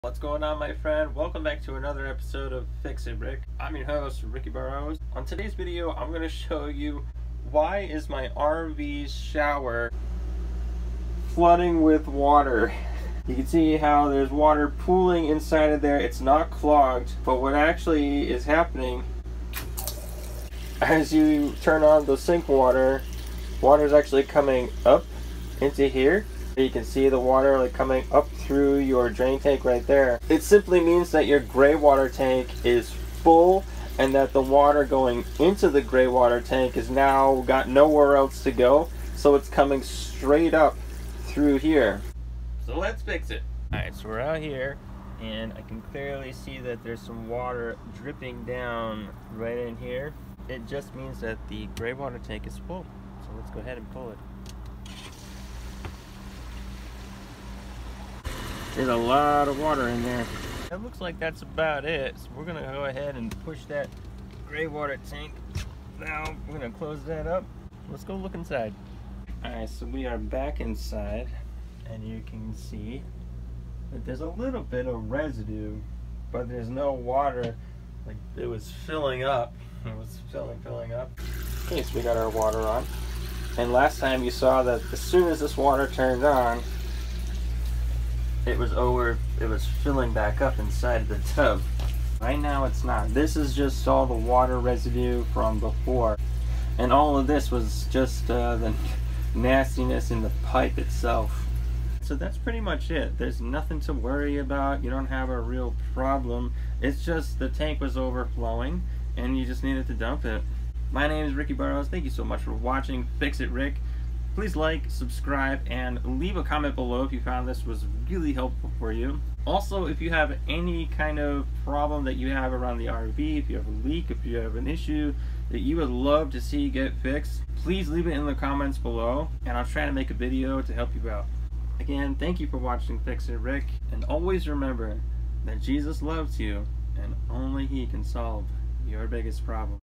RV Shower Backfilling? Easy Fix!
Love this channel?

Do you see water coming UP through your RV shower drain? Thank God there's a simple solution for that! Your gray water tank is probably full and needs to be drained out. This typically happens after having a shower and washing several dishes multiple days in a row. Hope this video helps!